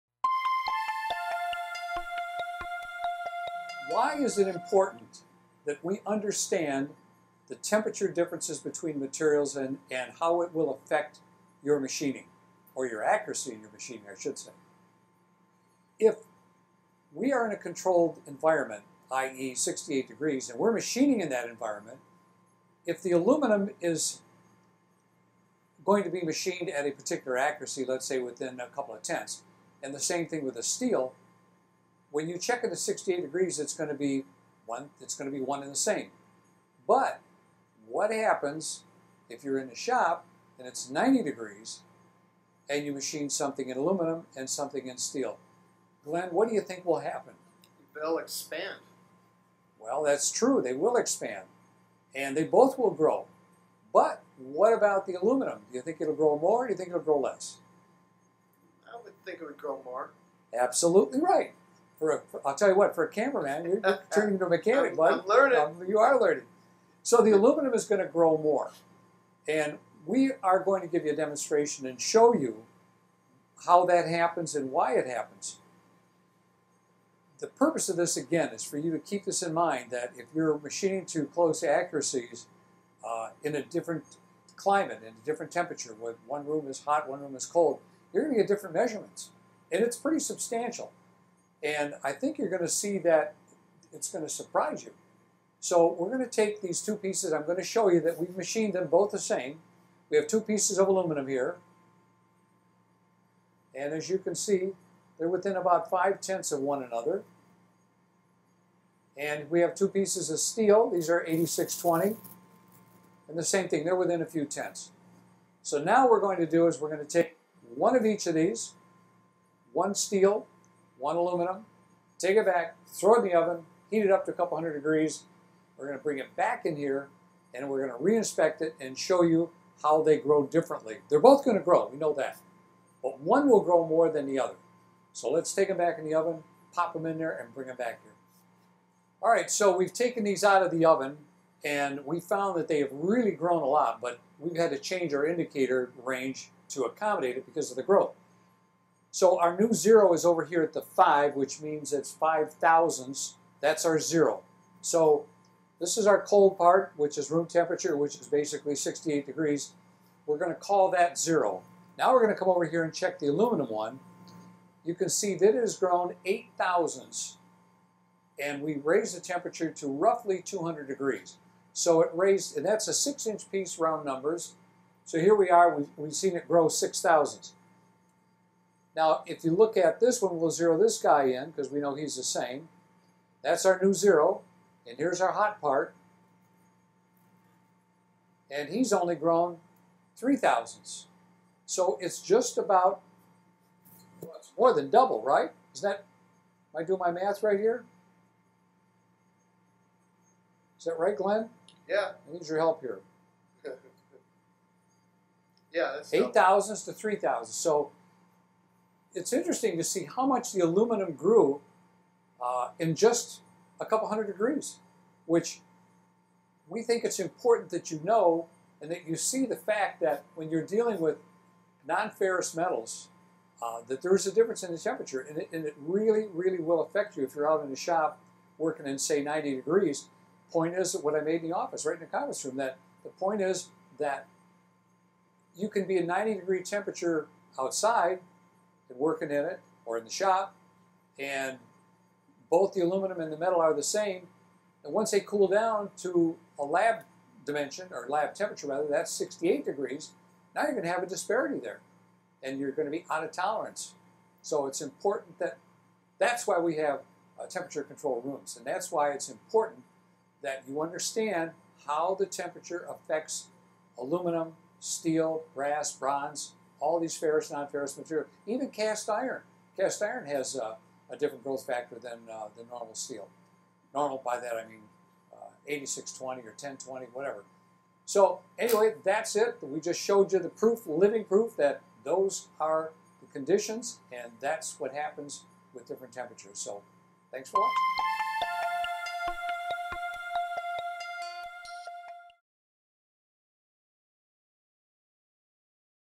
Why does temperature matter when checking dissimilar materials?
Steel and aluminum do not expand at the same rate. Take a look and see what happens when we add heat and measure them.